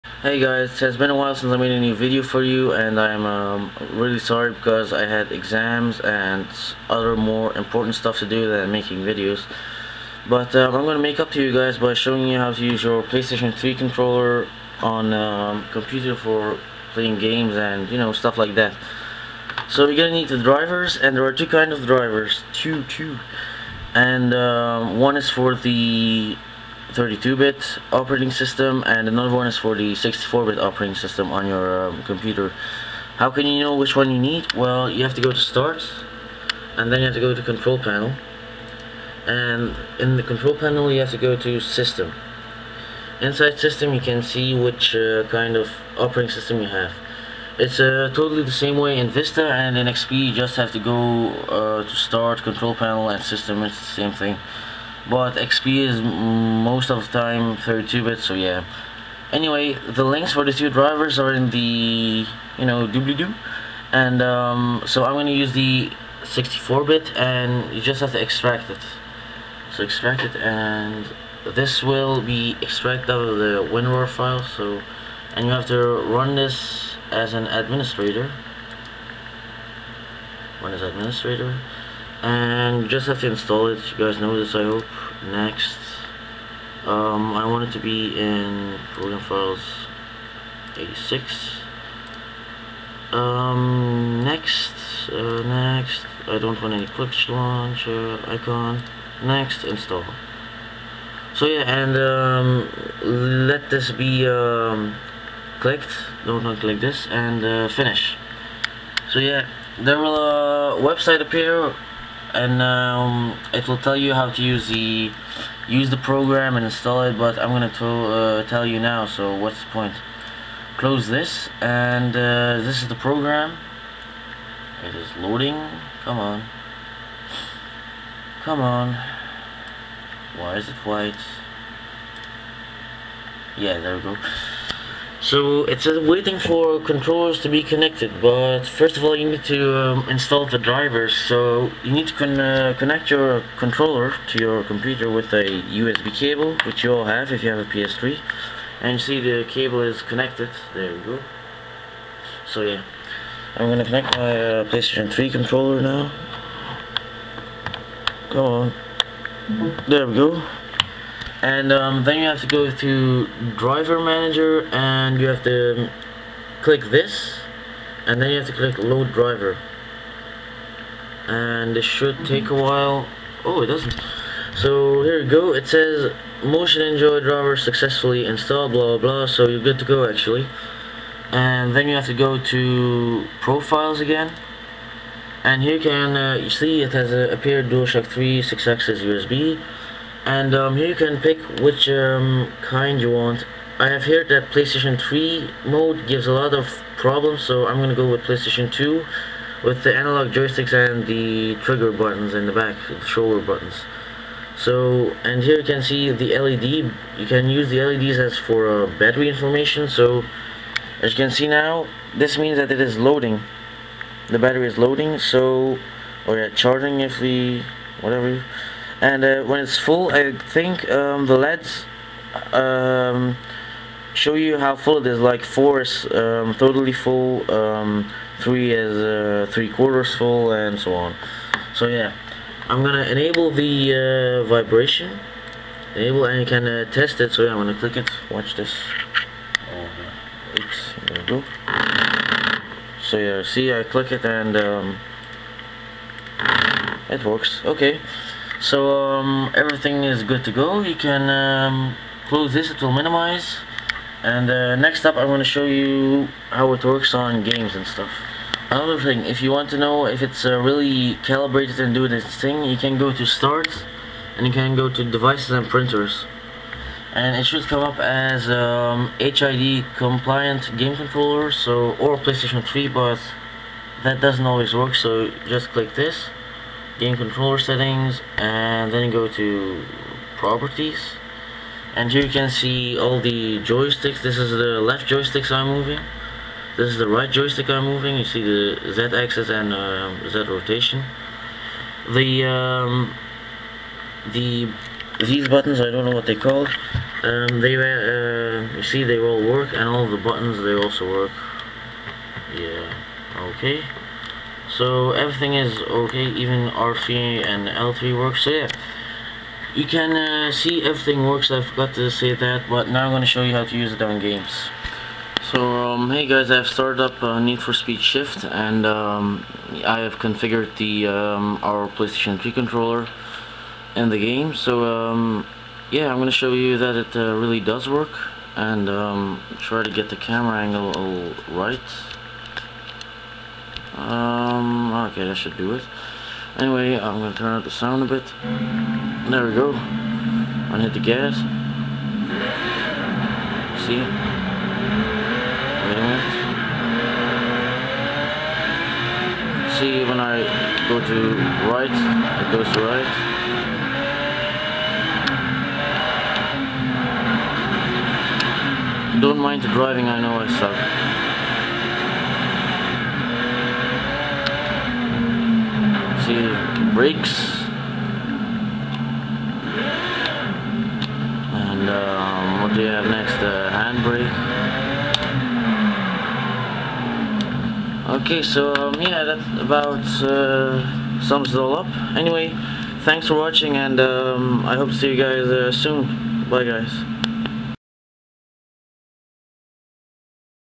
How to use a PS3 controller on your PC
In this video I'll show you how to use a PS3 controller on your computer for playing games and stuff.

If the above links do not work the driver is out of date.

And get the latest driver.